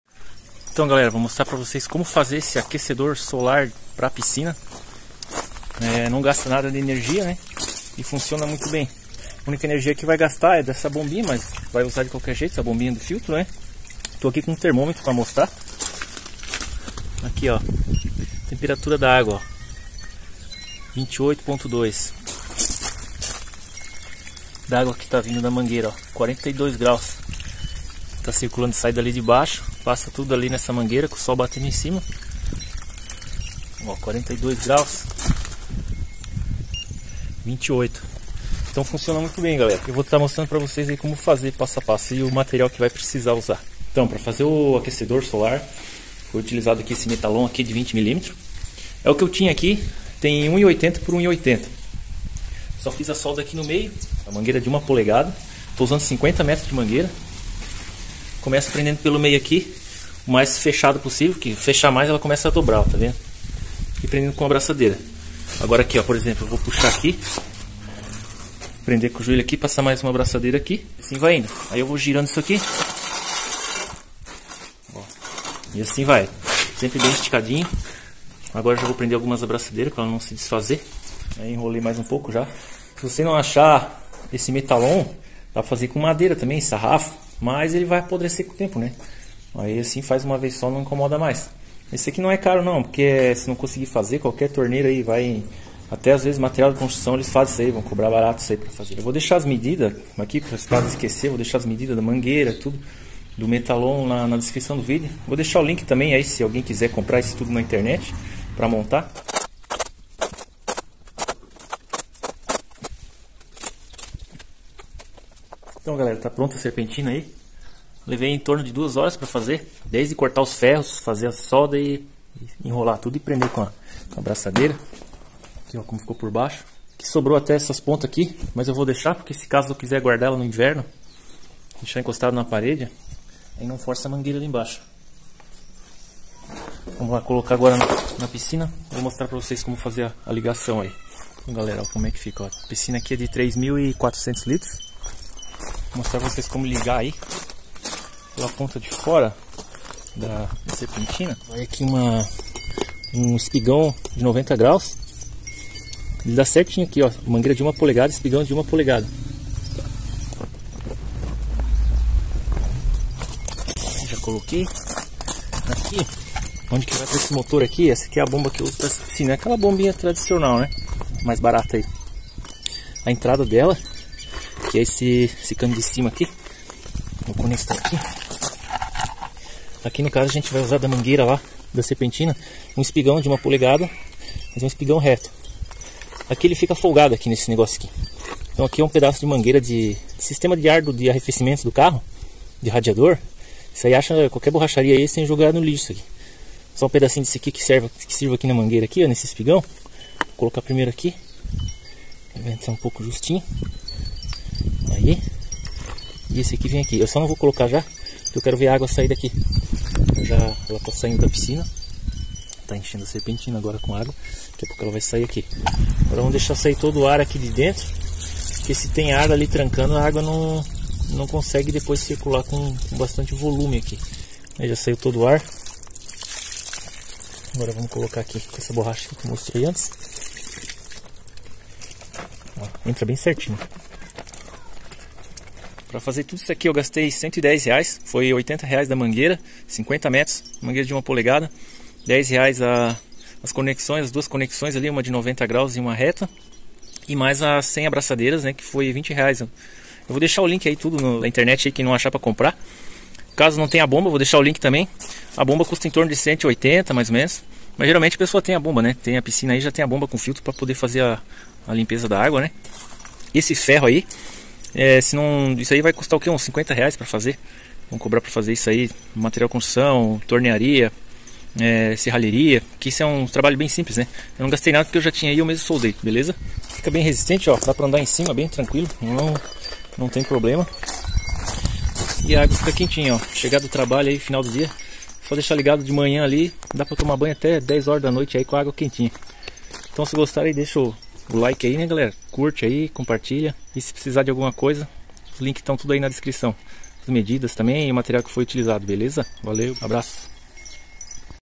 Como fazer Aquecedor Solar para Piscina - Fácil e Barato!
MATERIAIS UTILIZADOS:
2 metalon 20mm X 1,80 metros
50 Metros de mangueira de irrigação de 1 Polegada,
75 Abraçadeiras de nylon 4,8mm X 200mm,
1 União de 1 Polegada p/ mangueira
1 Cotovelo de 1 Polegada p/ mangueira